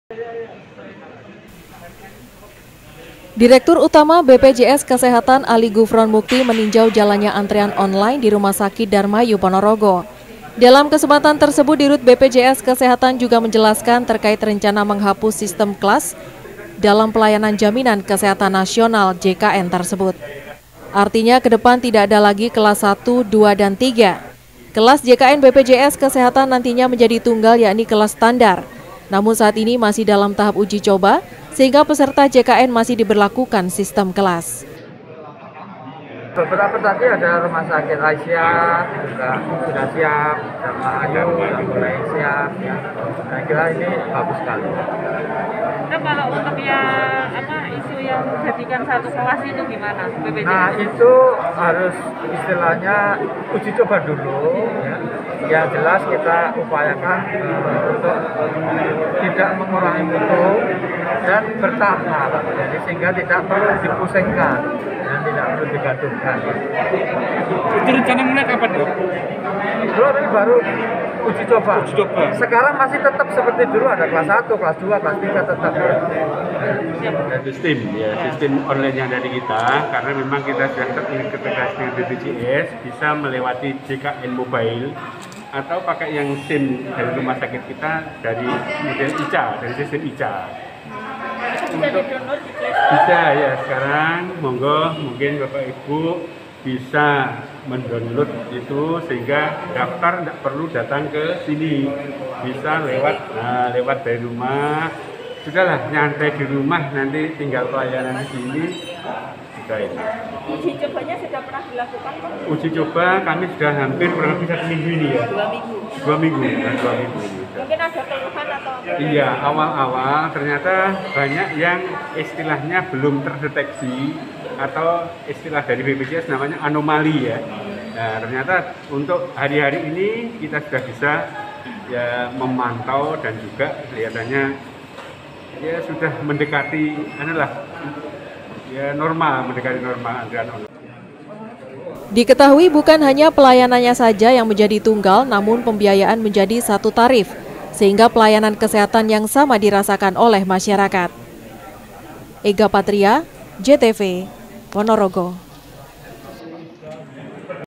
PONOROGO - DIRUT BPJS Kesehatan : Rencanakan Penghapusan Golongan Peserta JKN
Direktur utama bpjs kesehatan ali gufron mukti meninjau program antrean online bpjs di beberapa rumah sakit di ponorogo. Selain itu bpjs kesehatan juga berencana merubah golongan kelas untuk disama ratakan menjadi golongan standar.